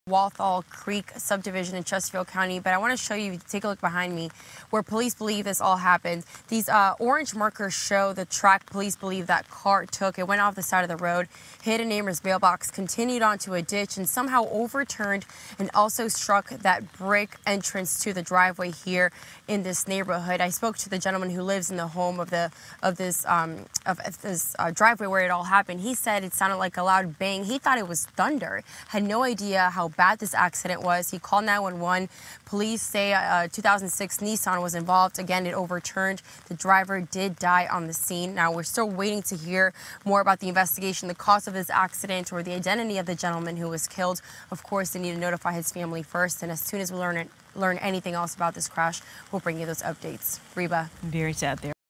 Car Flips, Driver Dies in Overnight Chesterfield Crash
The crash occurred in the 1900 block of Walthall Creek Drive, around 12:26 a.m.  Monday, July 4.